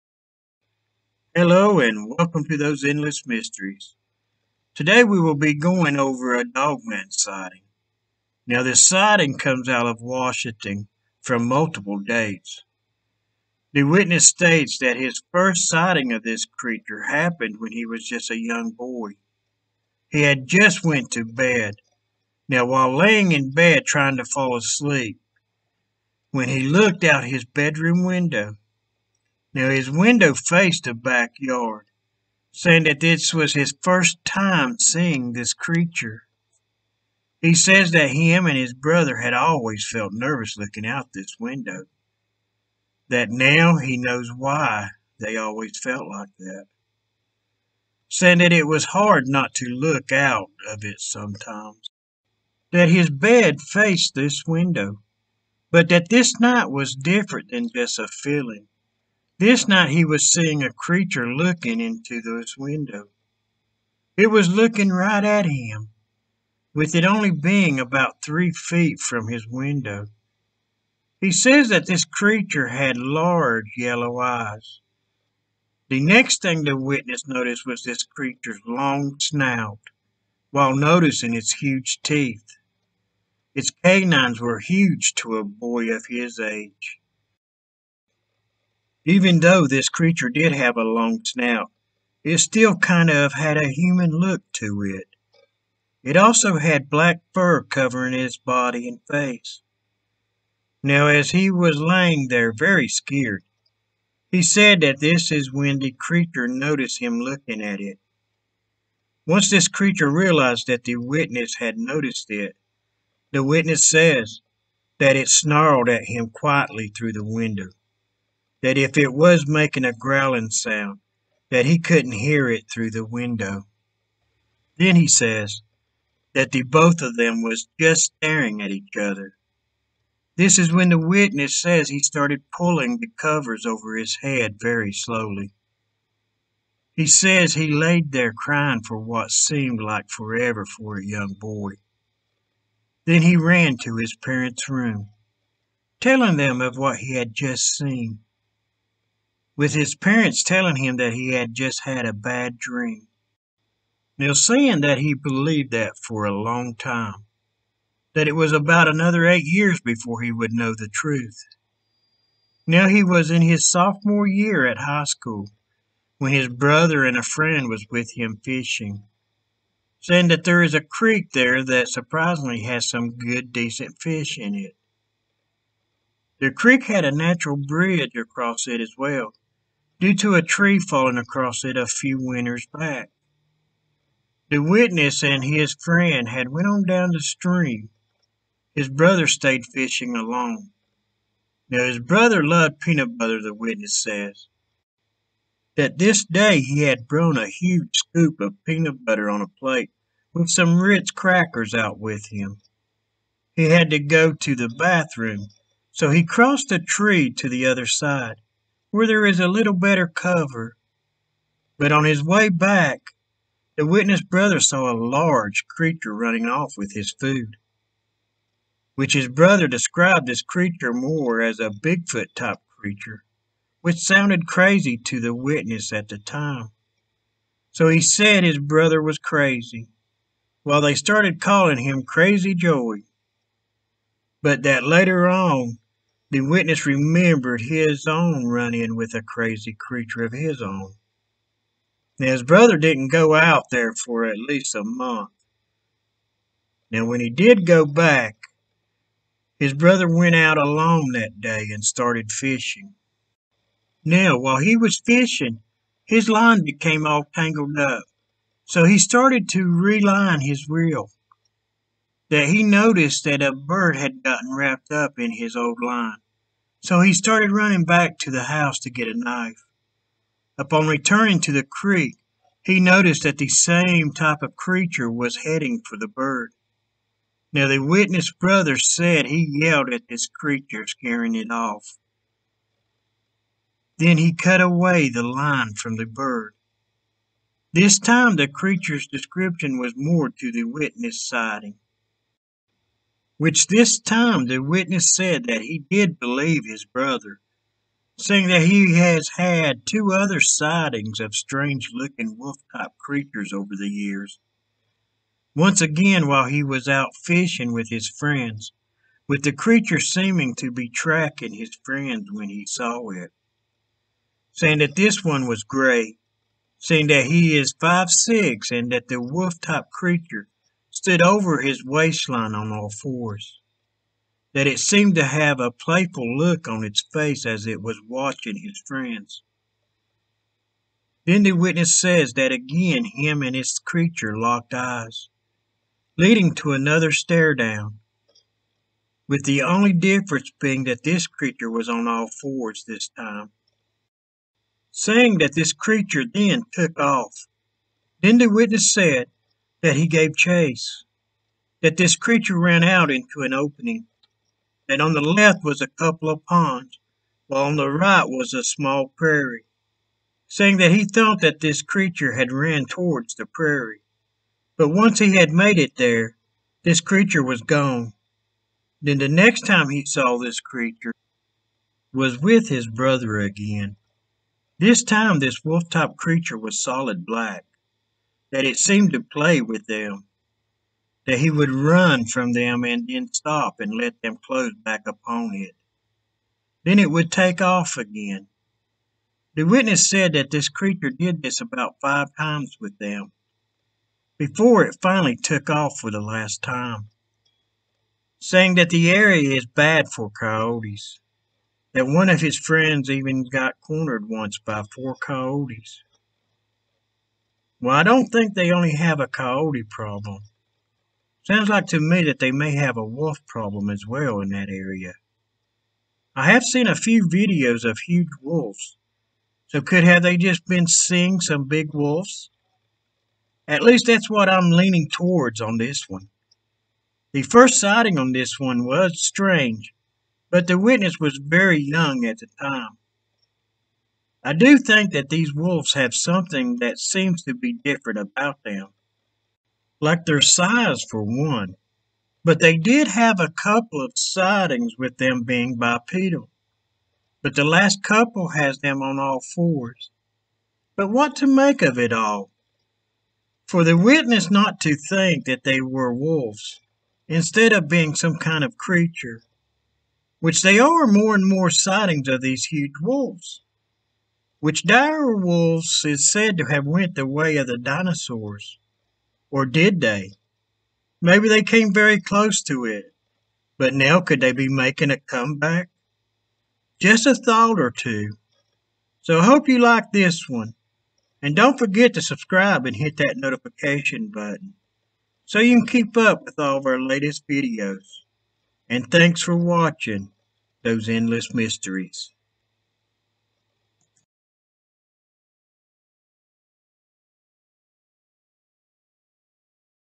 A Dogman Sighting From Washington
Join Those Endless Mysteries as we take a look into a Dogman sighting. This Sighting report comes out of Washington.